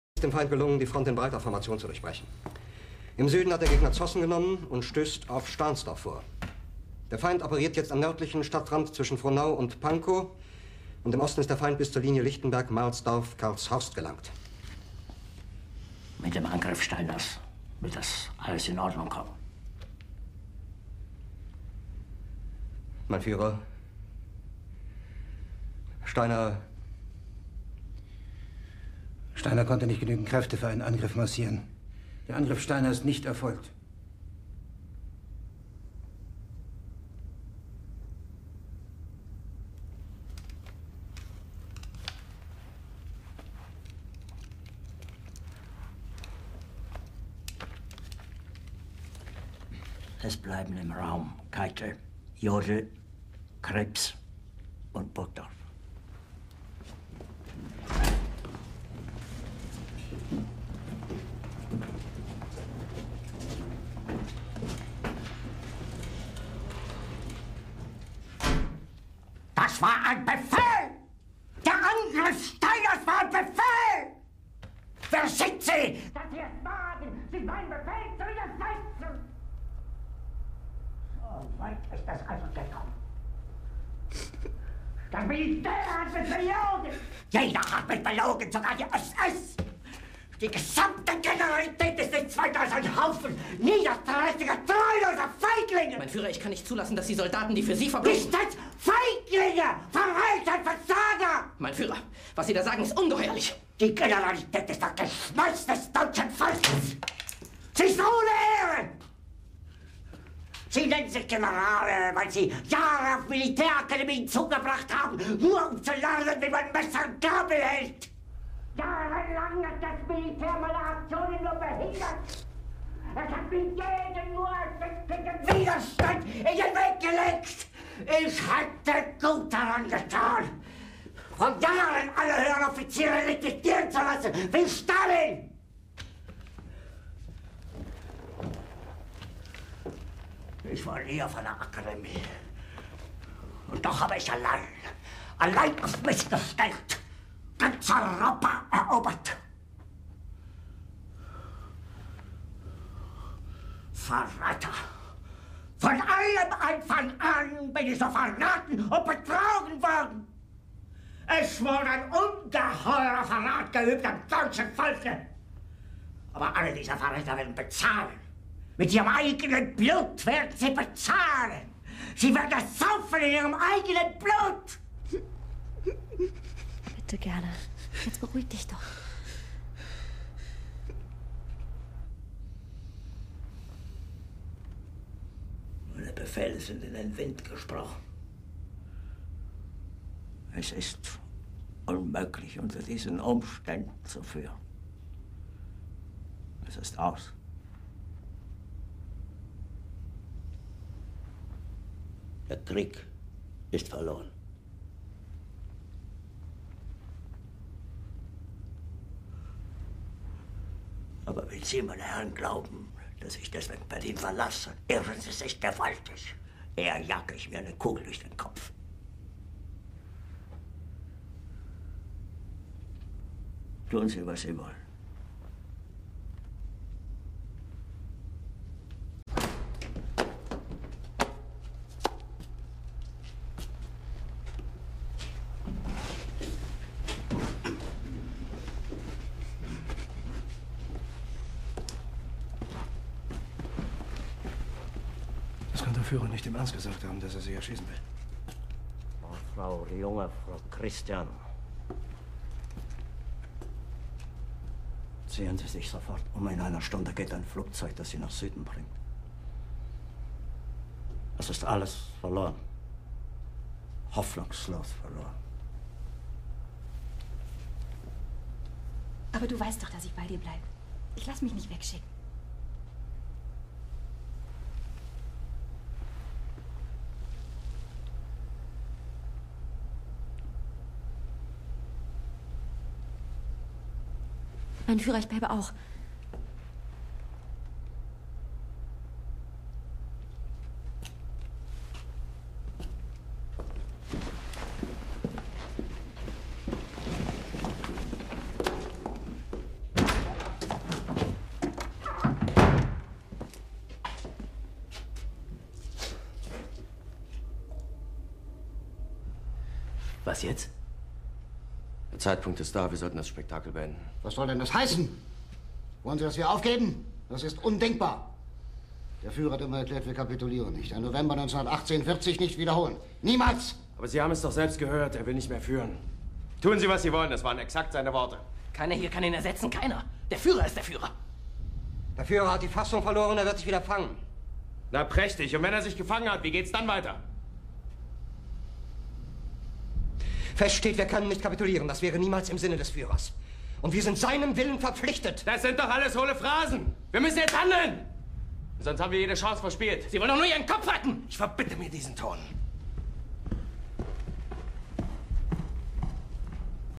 Hitler reacts the JBrothersNUMBR1fan video entitled "AC/DC SUCKS!"
Hitler's favorite band is AC/DC, so when the haters says AC/DC sucks, I send a team of SS to send you in a gas chamber in Auschwitz.